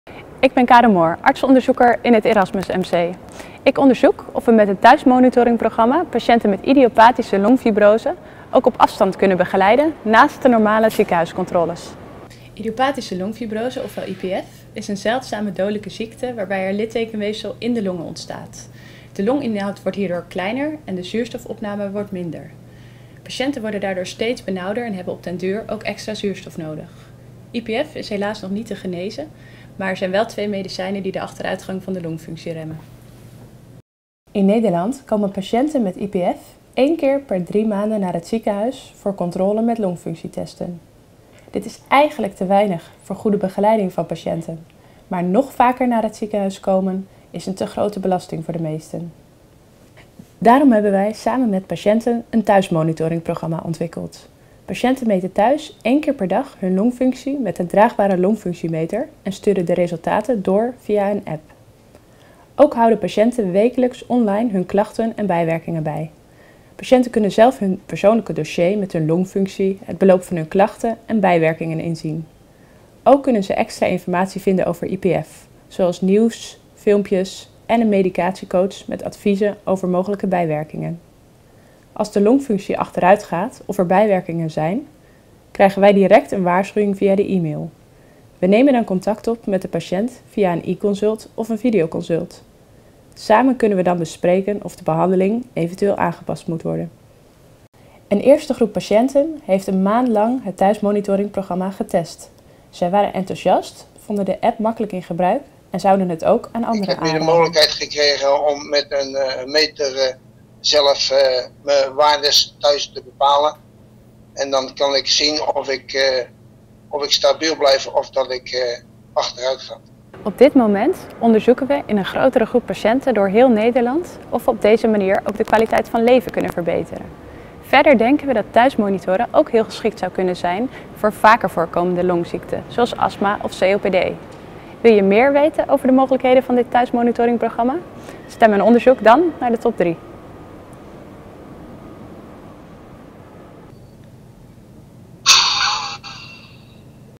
De gezichten van het Erasmus MC - Karen Moor
Arts-onderzoeker Karen Moor vertelt over haar onderzoek. Thuis je longfunctie meten en online klachten bijhouden. Zo kunnen mensen met idiopathische pulmonale fibrose (IPF) sneller worden geholpen zonder direct naar het ziekenhuis te hoeven en krijgen zij een actievere rol in het monitoren van hun ziekte. U kunt haar onderzoek promoten door te stemmen op de website van het longfonds